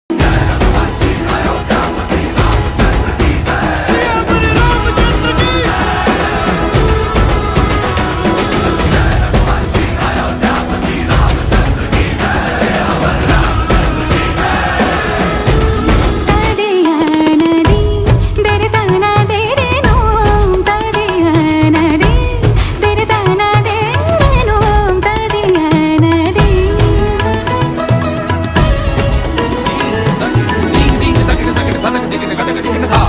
Complete Shree Ram Lalla Sketch 😍✨️
Shree Ram Lalla colour pencil sketch ✨️

Working on 55 hours

Hyper Realistic colour pencil sketch 😍